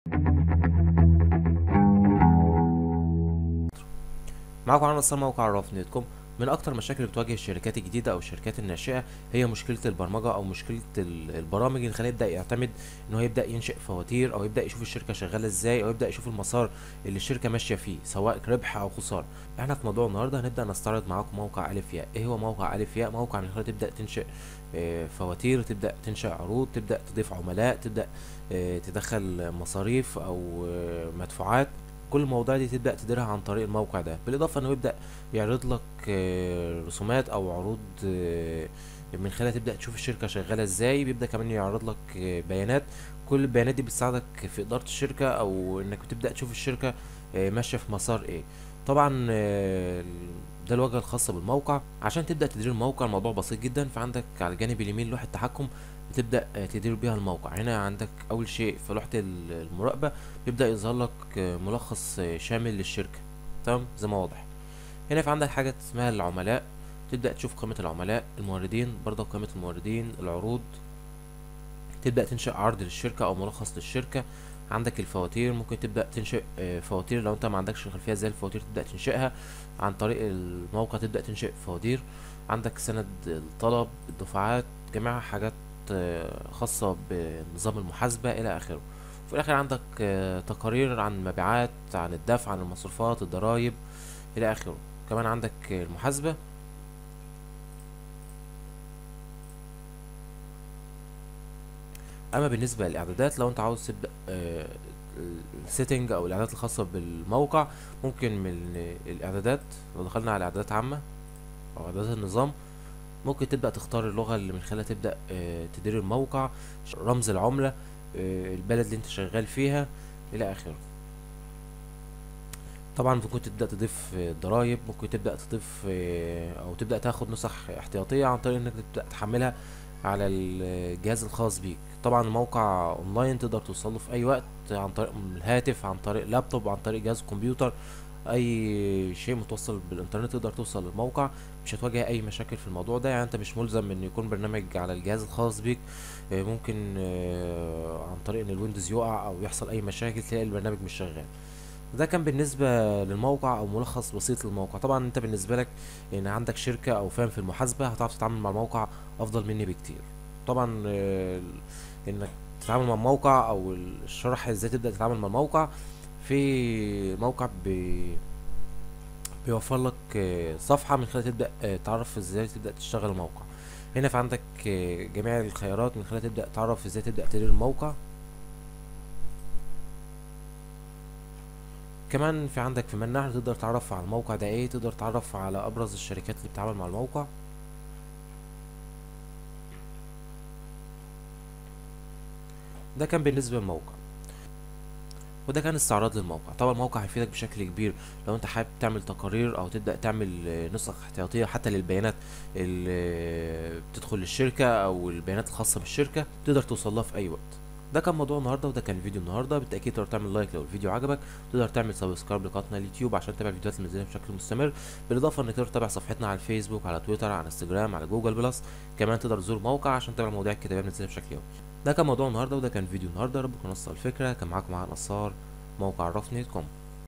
استعراض موقع aliphia لإنشاء فواتير و ادارة الأمور المحاسبية الخاصة بالشركات
من أكثر المشاكل التي تواجه الشركات الناشئة او المشروعات الجديدة والتي تعتمد علي نظام المحاسبة من أرباح وخسارة, هي فكره وجود برنامج من خلالة يتم اعطاء تقارير عن شركه او المشروع وعن المسار الذي يسلكه, فا تكلفه برمجة هذه البرامج عالية وكثير من الشركات لا تسطيع تغطيه مصروفاتها وايضا ضمان عملها بسبب احيانا قد تتعطل او قد يصاب النظام بضرر .. ألخ.